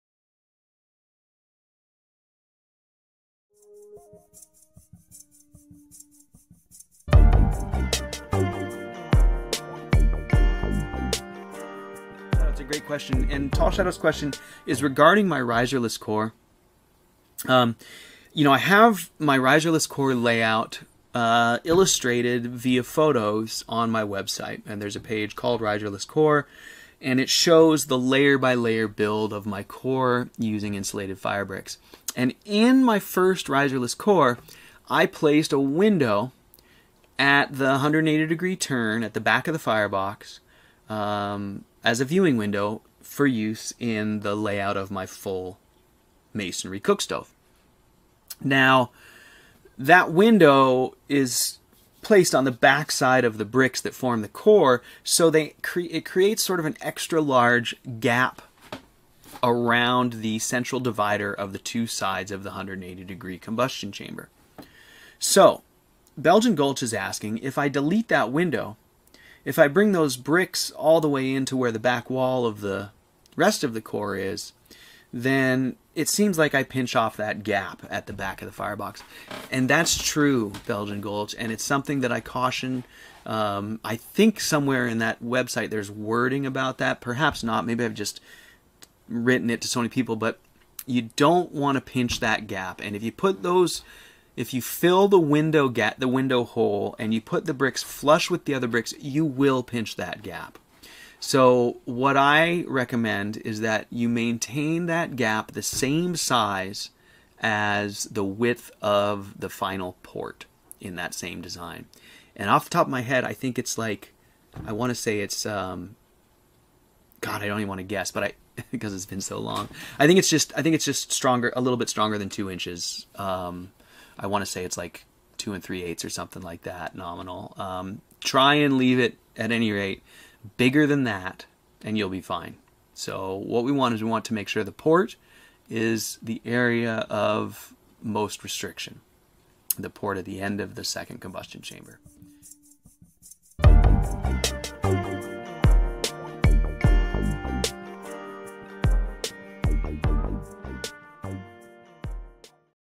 Walker Riserless Core Details Discussion
A discussion on some details of the Walker Stoves Riserless Core, and excerpt from Stove Chat Live Episode 23.